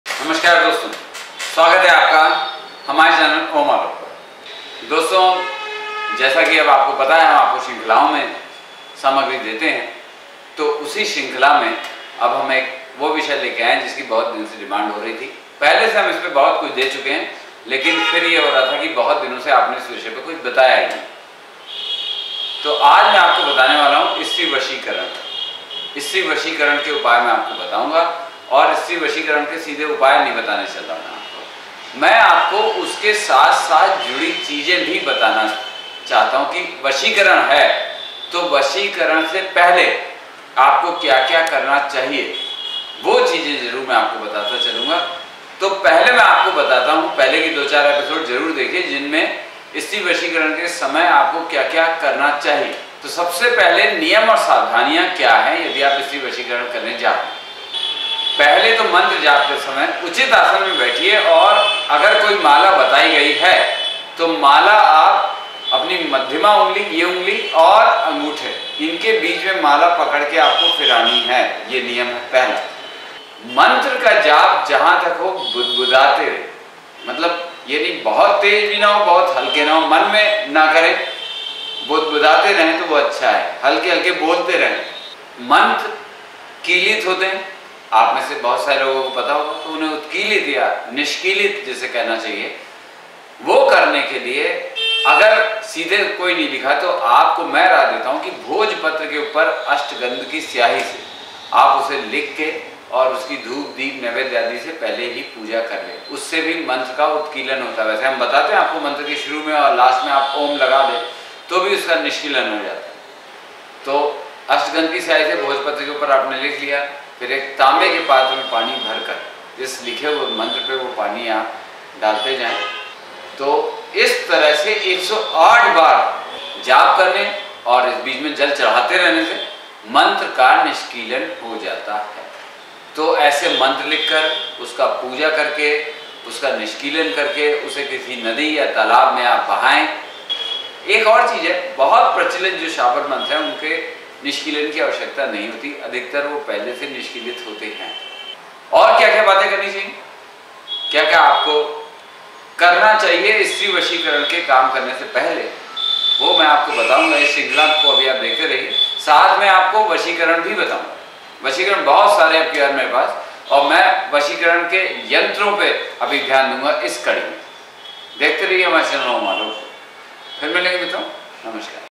स्त्री वशीकरण संधान - भाग १
स्त्री वशीकरण से सम्बंधित प्रयोगों में मंत्र जाप के समय उचित आसन में बैठ कर दाहिने हाथ की मध्यमा ऊँगली और अंगूठे से हीमाला को फिराएं और यदि हो सके तो मंत्र को बुदबुदाते हुए जाप करें. उत्कीलन(या निष्कीलन) के लिए जिस यंत्र का जप करना है, उसे पहले अष्टगंध द्वारा भोजपत्र पर लिख कर धूप, दीप, नैवेद्य आदि से पूजन करें,. फिर ताम्बे के एक पात्र में पानी भर कर उसे भोजपत्र पर लिखे मंत्र पर डालते जाएँ, साथ ही मंत्र भी जपते जाएँ. इस प्रकार मन्त्रों पर जल चढ़ाने और १०८ बार मन्त्र जाप करने से यन्त्र का उत्कीलन हो जाता है.
इस विधि से मंत्र लिख कर पूजन आदि करके मंत्र लिखे हुए भोजपत्र को किसी नदी तालाब आदि में बहा देने से भी मंत्र का उत्कीलन हो जाता है.बहुत अधिक प्रचिलित शाबर मन्त्रों के उत्कीलन की आवश्यकता नहीं होती.